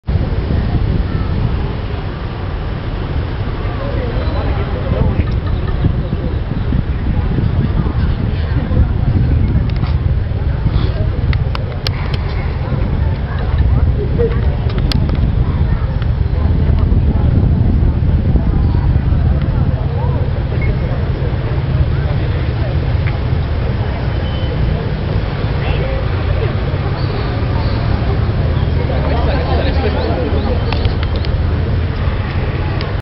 Palermo, maxi tamponamento fra 8 vetture fra via Lazio e via Sicilia
Cinque feriti e otto auto coinvolte,tra cui un’auto della polizia. E’ questo il bilancio della maxi carambola che si è verificata in viale Lazio questo pomeriggio. Nelll’incidente sono rimaste coinvolte oltre alla vettura della polizia anche una Bora Volkswagen, una Ford Focus, una Smart, una Chevrolet e una Opel, una Seat e Hyunday. I cinque feriti non sono gravi, ma viale Lazio è stata chiusa al traffico.